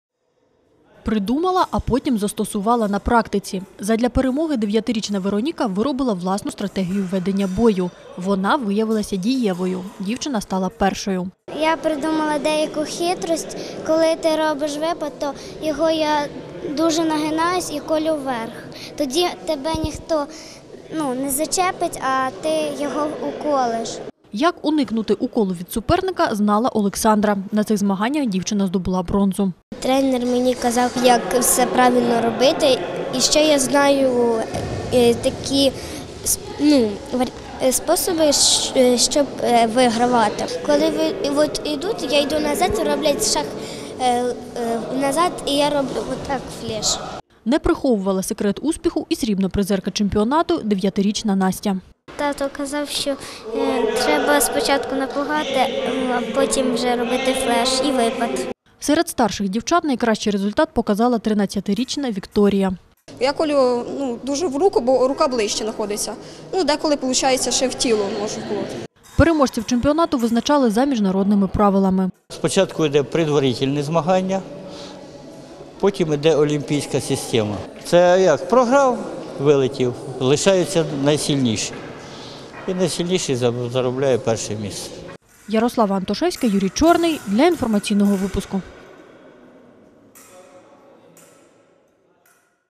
У Хмельницькому закінчилися обласні змагання з фехтування
Дев’ятнадцять років поспіль його присвячують герою Радянського Союзу Івану Філіпову. Чоловік загинув у мікрорайоні Гречани при виконанні військового обов’язку в 1944 році. Нині змагання зібрали 35 шпажистів.